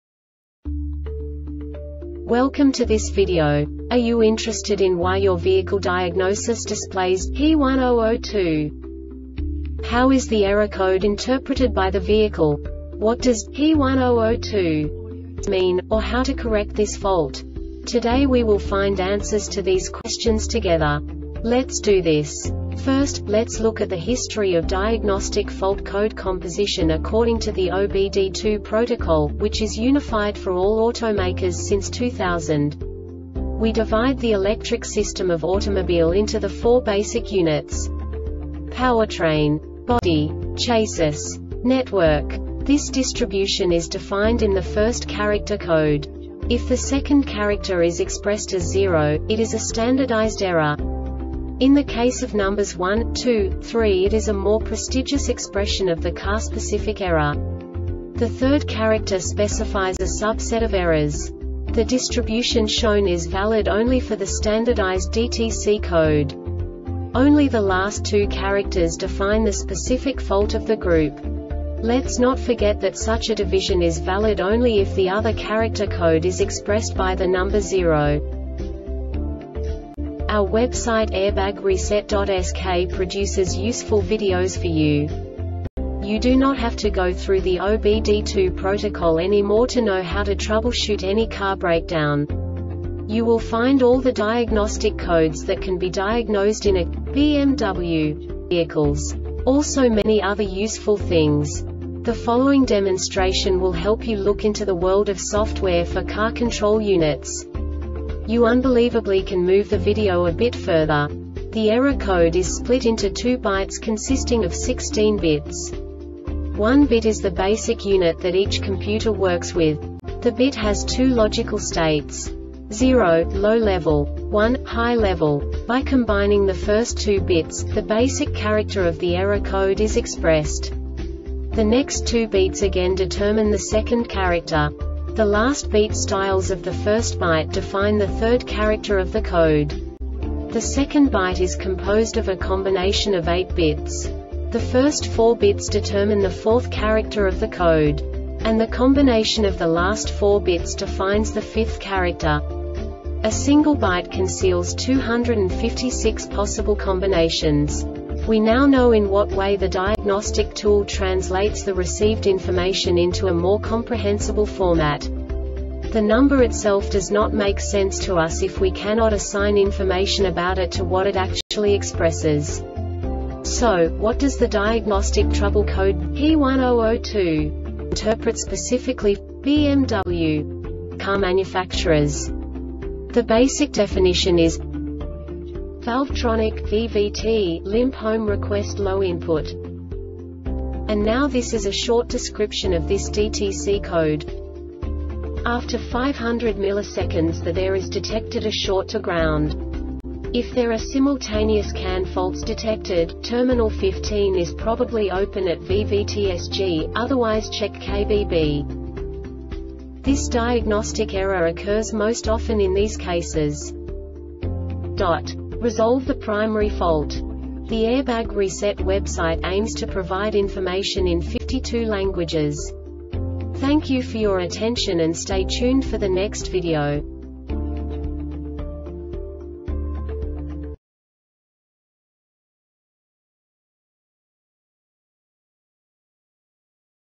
DTC BMW P1002 Short Explanation
Contents:
0:21 Basic DTC analysis according to OBD2 protocol standard.
1:48 Insight into programming
2:58 A diagnostically understandable description of the Diagnostic Trouble Code
3:05 Basic error definition

Make:
BMW
Code:
P1002
Definition:
Valvetronic (VVT) Limp Home Request Low Input
Description:
After 500ms the there is detected a short to ground. If there are simultaneous CAN faults detected, Terminal 15 is probably open at VVT-SG, otherwise check Kb-B.
Cause:
Resolve the primary fault.